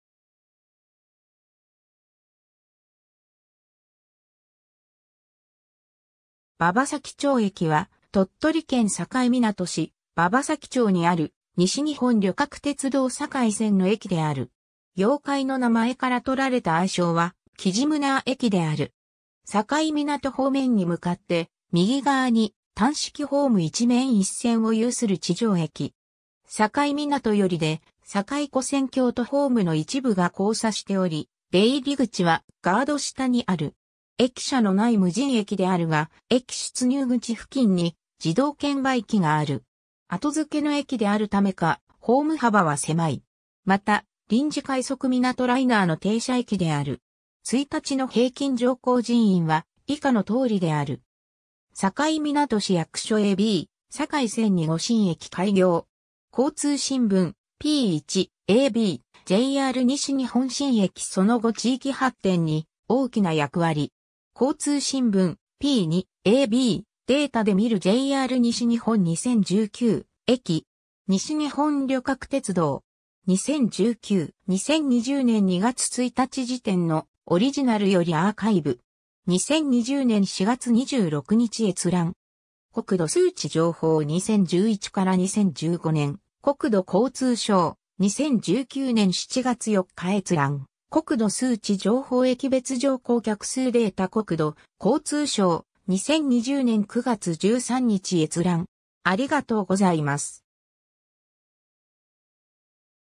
馬場崎町駅
馬場崎町駅（ばばさきちょうえき）は、鳥取県境港市馬場崎町にある、西日本旅客鉄道（JR西日本）境線の駅である。
妖怪の名前から取られた愛称はキジムナー駅である。
 境港方面に向かって右側に単式ホーム1面1線を有する地上駅（停留所）。
境港寄りで境跨線橋とホームの一部が交差しており、出入口はガード下にある。
駅舎の無い無人駅（米子駅管理）であるが、駅出入口付近に自動券売機がある。
後付けの駅であるためか、ホーム幅は狭い。
 また、臨時快速みなとライナーの停車駅である。
 1日の平均乗降人員は以下の通りである。
 境港市役所 ^ a b “境線に5新駅開業”. 交通新聞 (交通新聞社): p. 1. (1987年10月17日) ^ a b “JR西日本 新駅その後 地域発展に大きな役割”. 交通新聞 (交通新聞社): p. 2. (1996年11月11日) ^ a b "データで見るJR西日本2019：駅" (PDF). 西日本旅客鉄道. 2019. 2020年2月1日時点のオリジナル (PDF)よりアーカイブ。
2020年4月26日閲覧。
 ^ 国土数値情報(駅別乗降客数データ)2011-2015年 - 国土交通省、2019年7月4日閲覧 ^ 国土数値情報　駅別乗降客数データ - 国土交通省、2020年9月13日閲覧